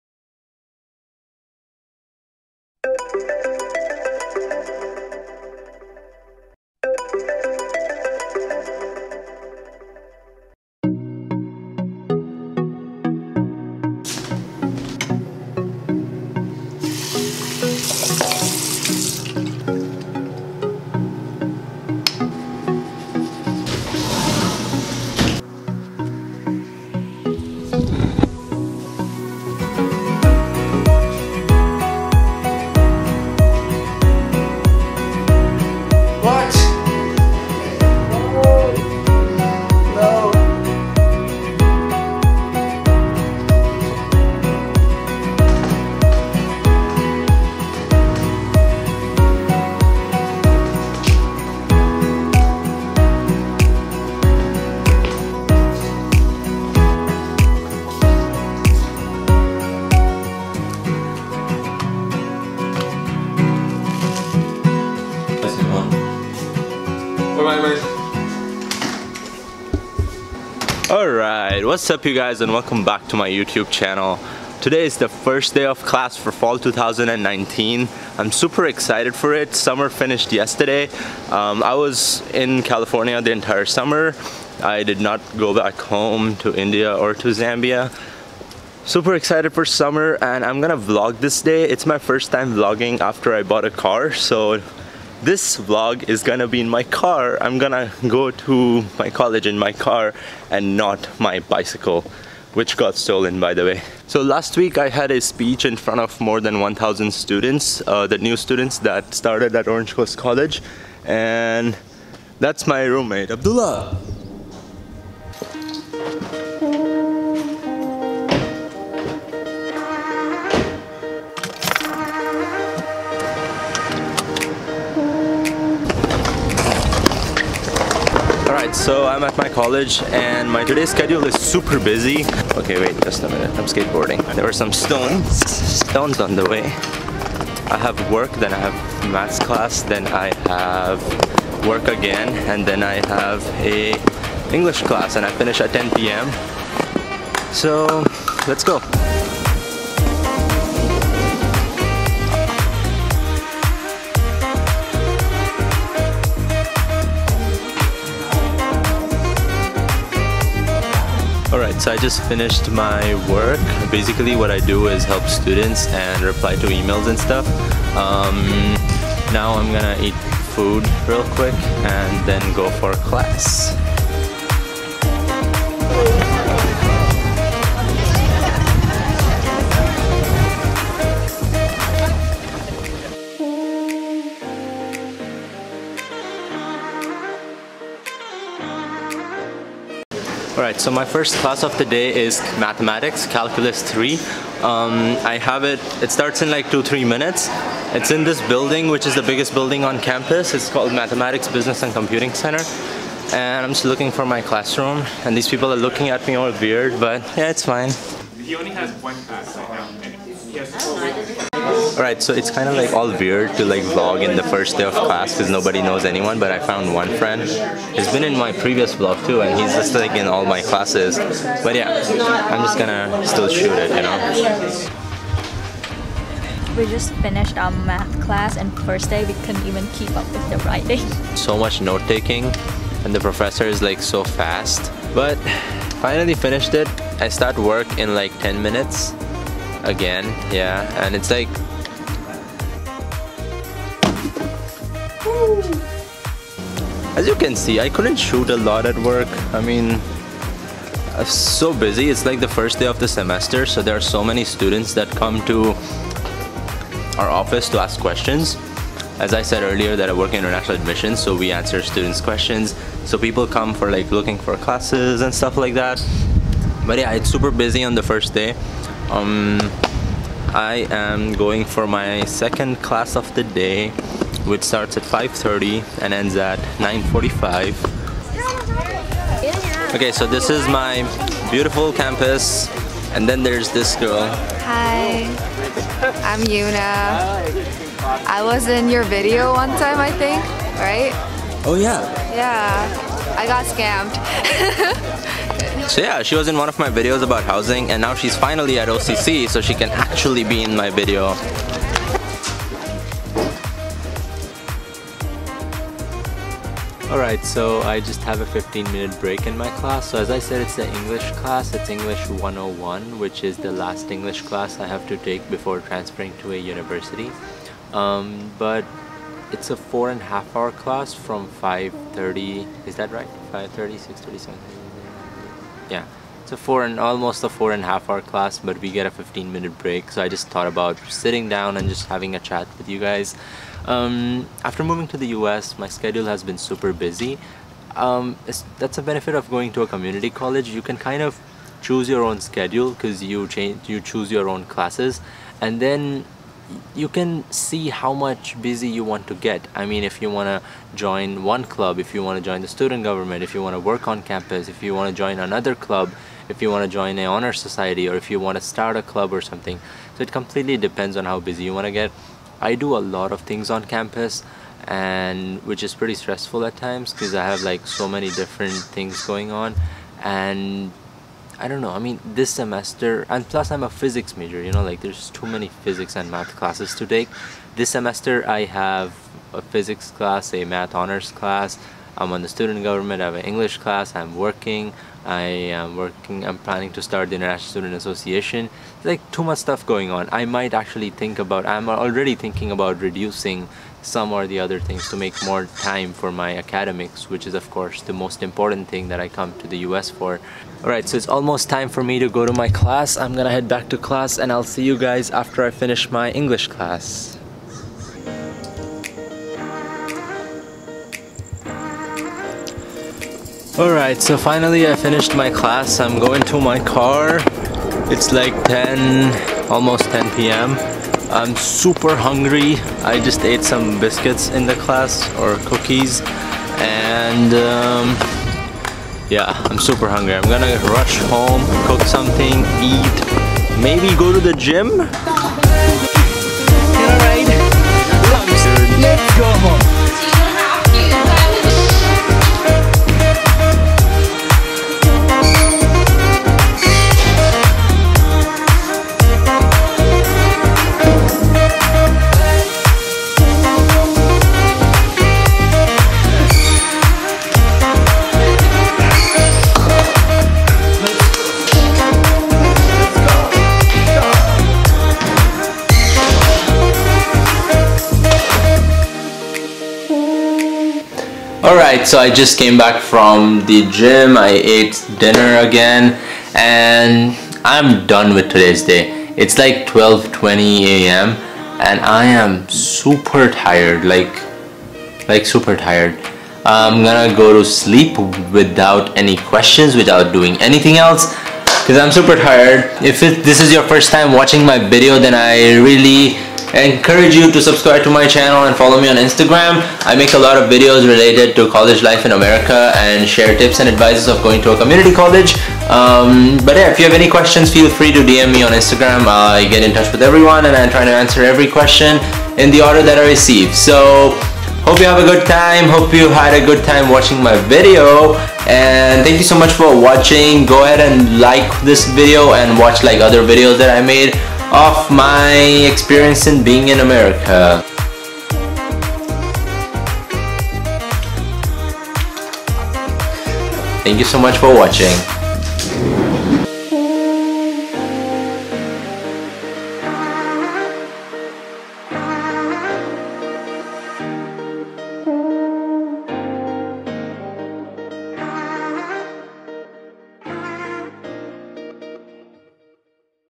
Day In The Life Of An OCC International Student | Nand Javia
Hellooo everyone! here is another day in my life video. I have been super busy these days and haven't had time to shoot and edit videos. However, if you have any suggestions please let me know I will try to make those videos. Let me know what you think about this video in the comments section below.